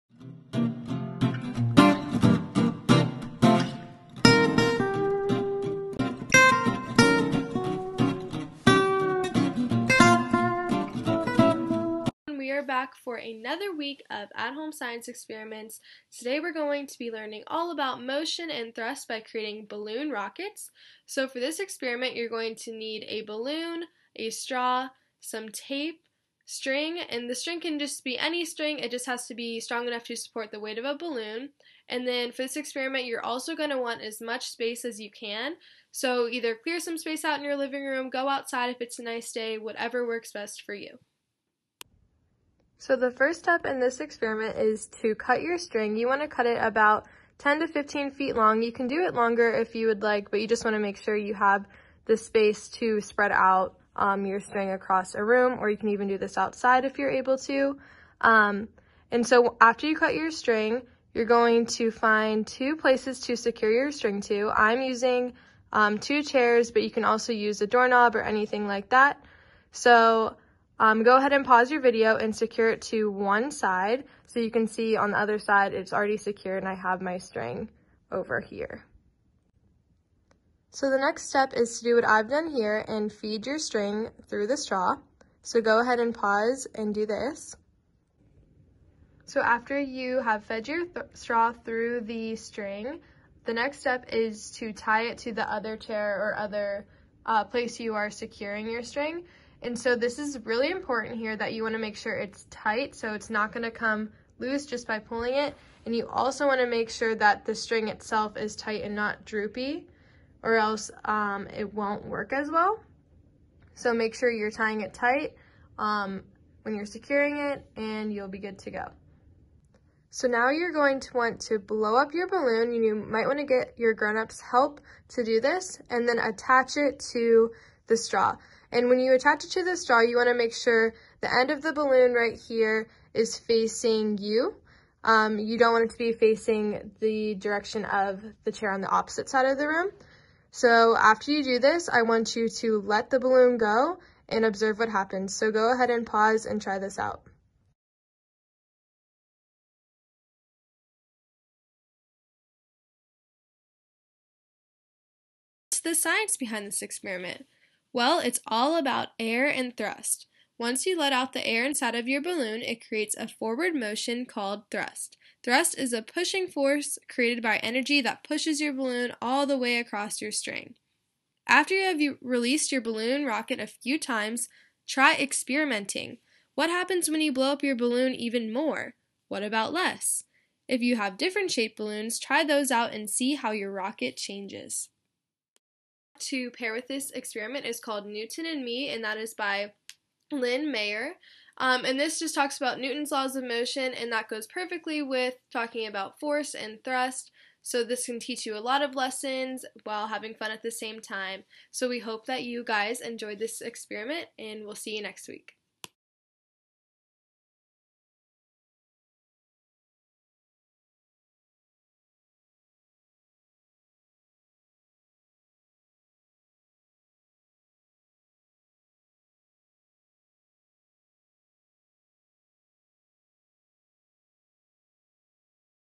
Science Story Time: Balloon Rocket
Participate in Penn State Behrend's popular Science Story Time—from home! This experiment will help children explore and describe the motion of toys and objects.